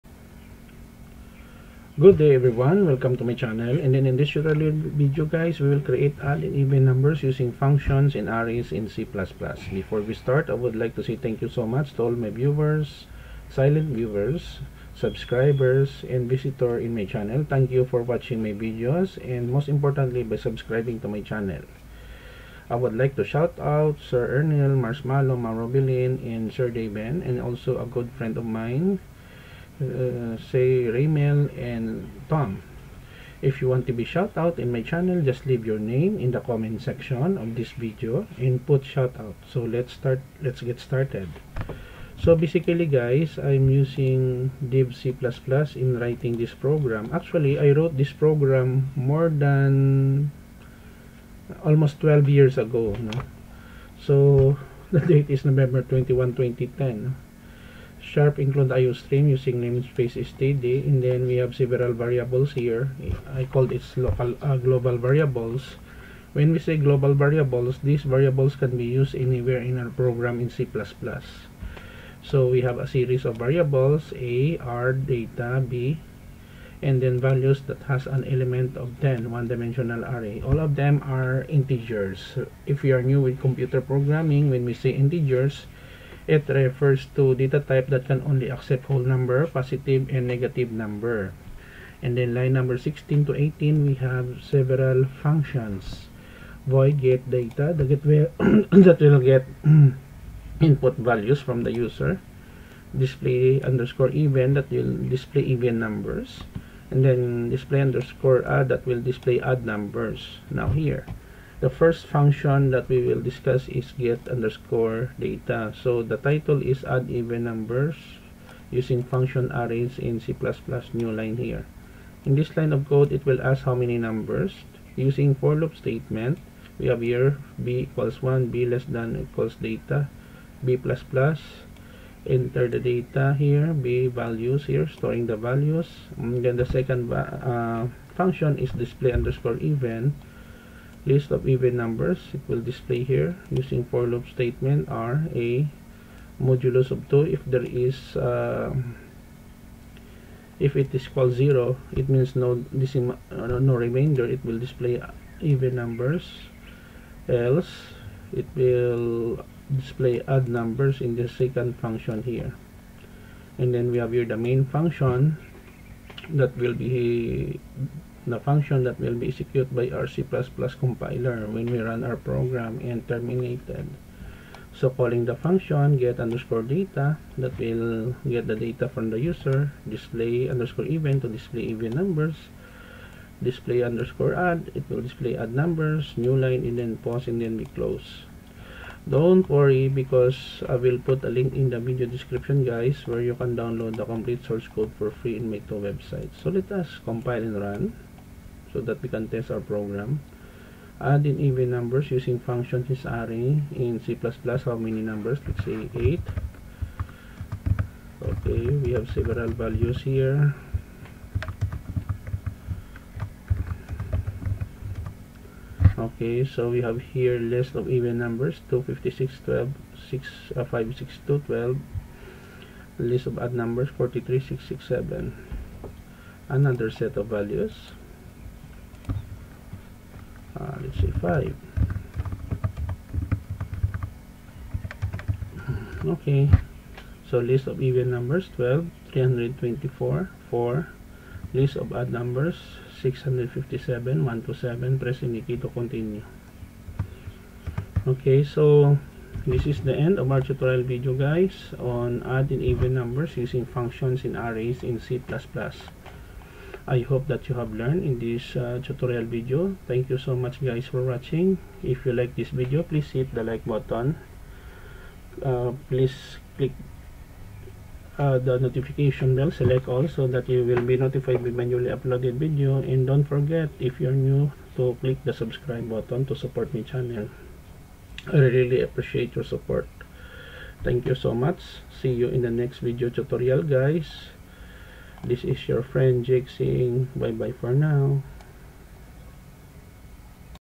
Odd and Even Numbers Using Functions and Arrays in C++
A simple program asks the user to give a series of numbers and then the program will display the odd and even numbers using functions and arrays using a C++ programming language.

GCash Account

Jake Pomperada